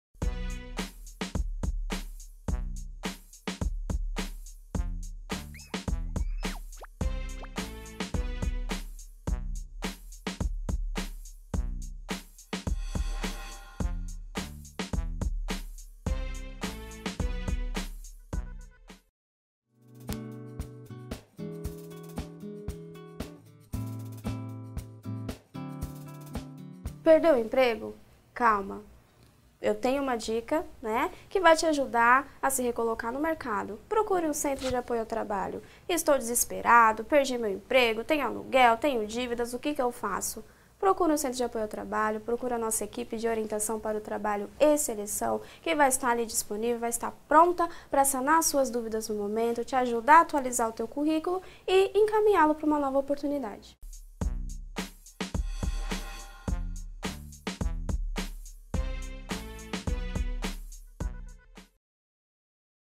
Dicas do CATe: Perdi meu emprego, e agora?
Graziela Vieira, líder de seleção dos CATe, dá dicas sobre entrevistas de emprego, currículo e sobre os serviços oferecidos no Centro de Apoio ao Trabalho e Empreendedorismo da Prefeitura Municipal de São Paulo.